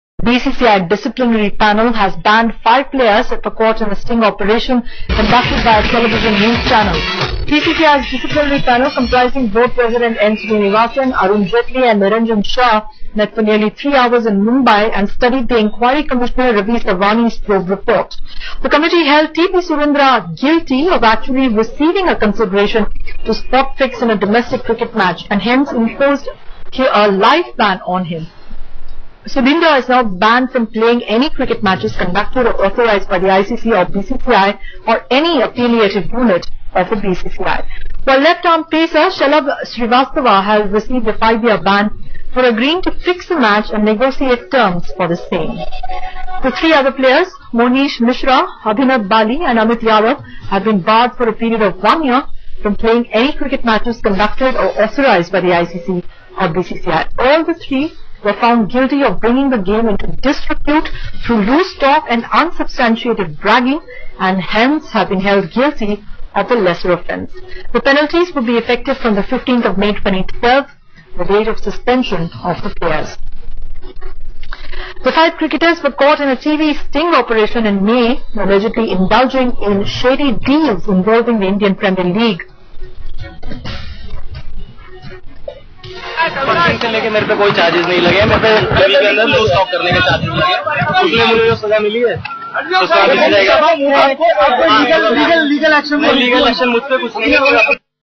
IPL: BCCI bans five players for spot fixing Part-2
Cracking the whip on cricketers caught in a IPL spot-fixing scandal, the BCCI on Saturday imposed a life ban on pacer T P Sudhindra but handed lighter punishments to four other domestic players who were also exposed in the TV sting operation.